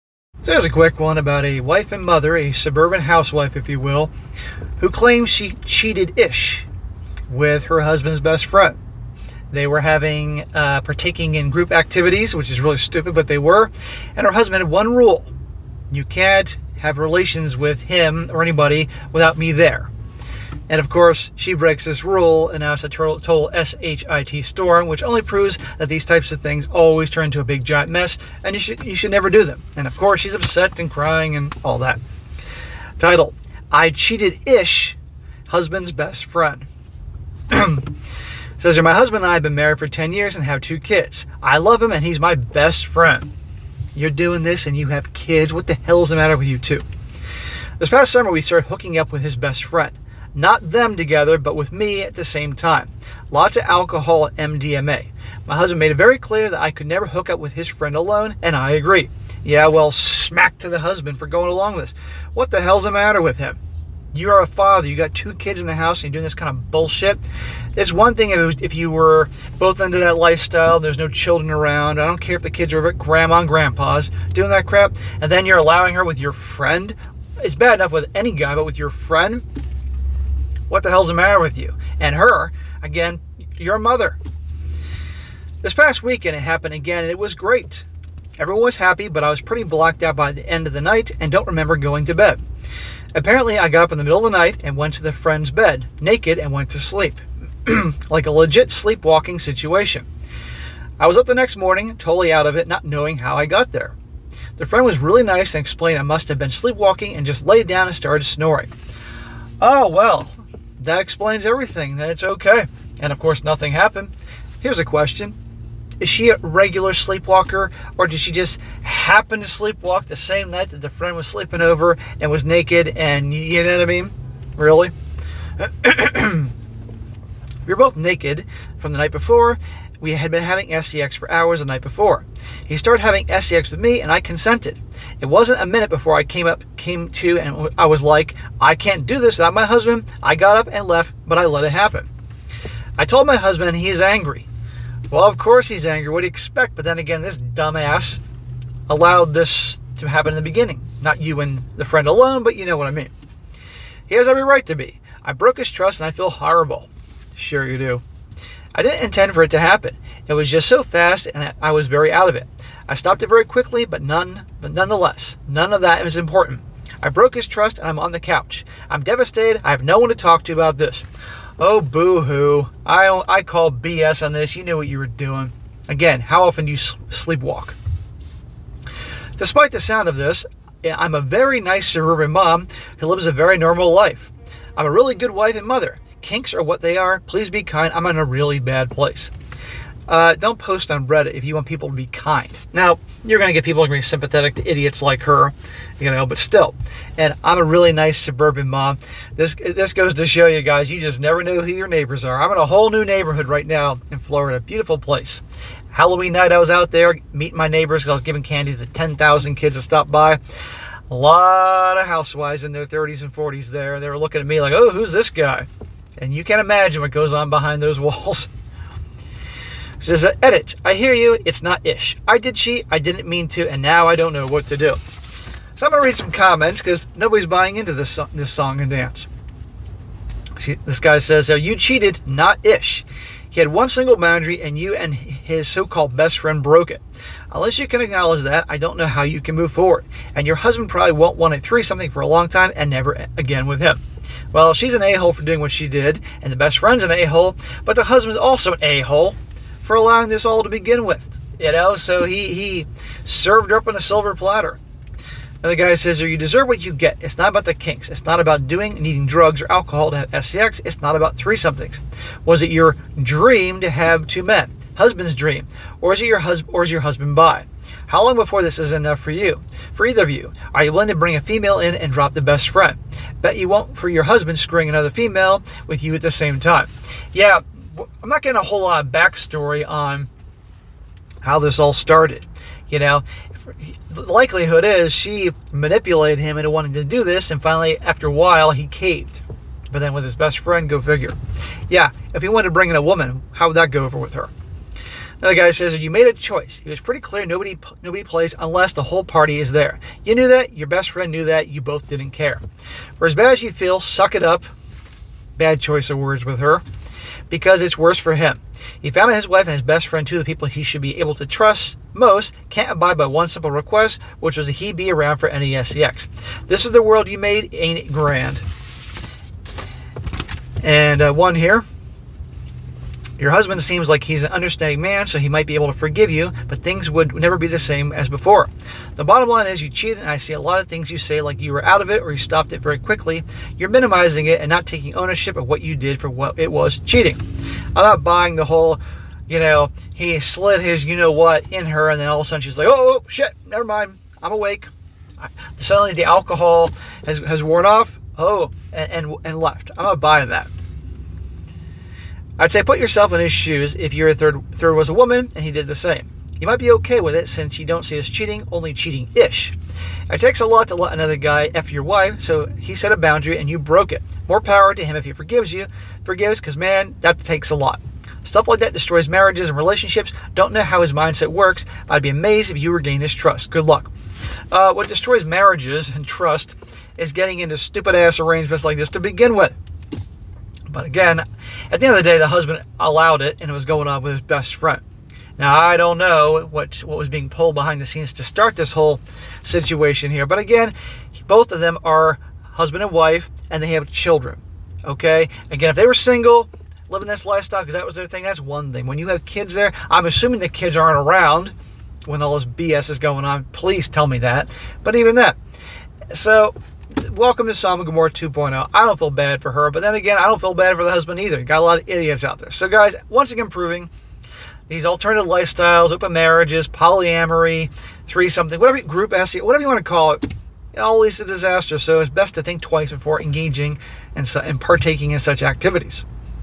Suburban Mum Claims She "Cheated-Ish" After Waking Up In Husband's Best Friend's Bed
SSM Clips

"Cheated-ish" huh?

SSM